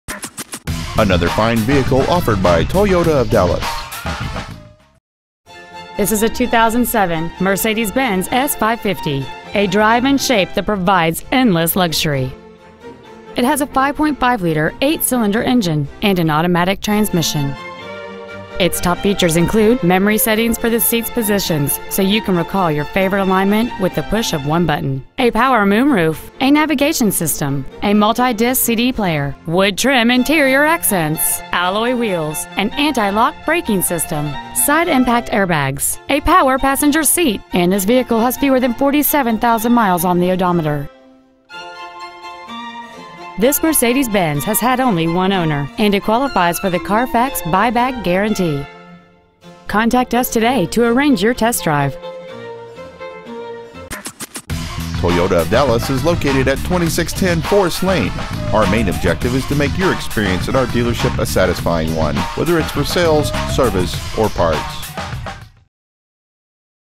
2007 Mercedes-Benz S550 Lewisville TX
Year: 2007
 Make: Mercedes-Benz
 Model: S550
 Engine: 5.5L 8 Cyl. Fuel Injected
 Trans.: Automatic
 Exterior: Pewter Metallic
 Miles: 46,715

 2007 Mercedes-Benz S550 Dallas TX